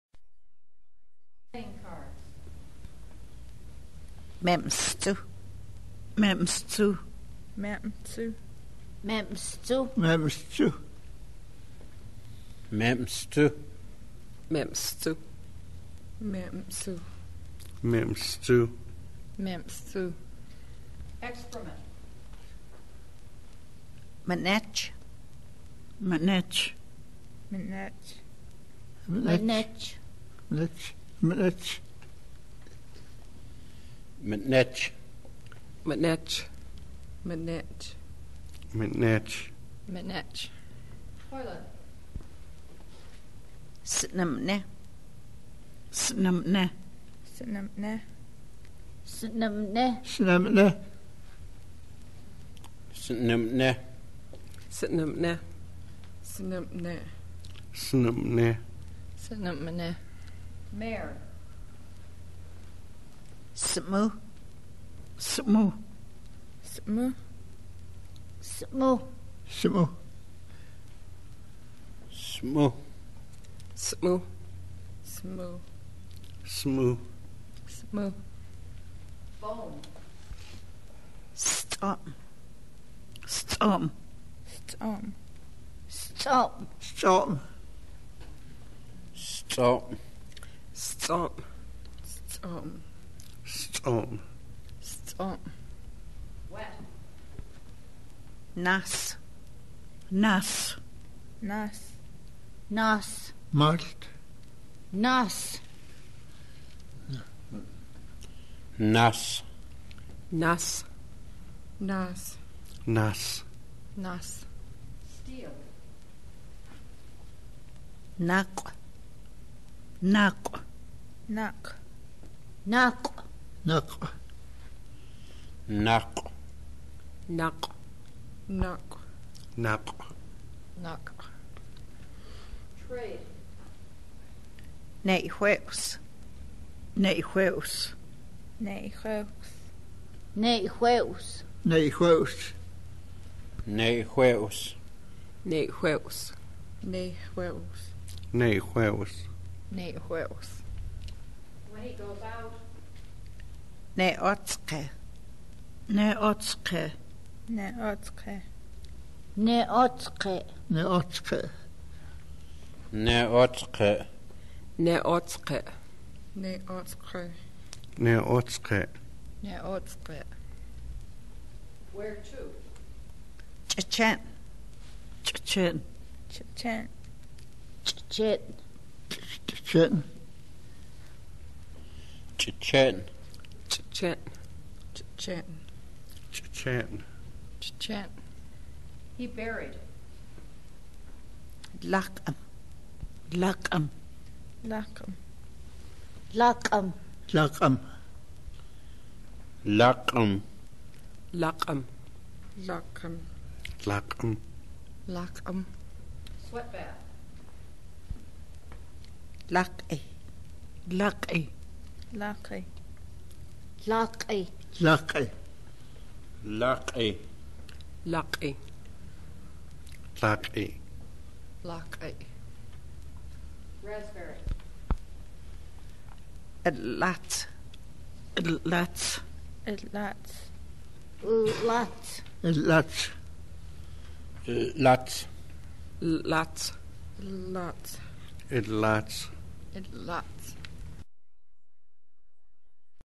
Flathead audio: fla_word-list_1992_29
Flathead (fla) audio: fla_word-list_1992_29
Language family: Salish; Interior Salish; Southern;
Flathead Cultural Center, Flathead Reservation, Montana
Date: 4/5/1992

Description: Language Montana Salish (Flathead).  fla_word-list_1992_01.html, enties: 57 - 69

Contributor: Harriet Whitworth, Felicite McDonald, Melanie Sandoval, Joe Eneas, Dorothy Felsman, Antoni (Tony) Incashola, Lucy Vanderburg, Marie Torosian, Clarence Woodcock, and Gloria Whitworth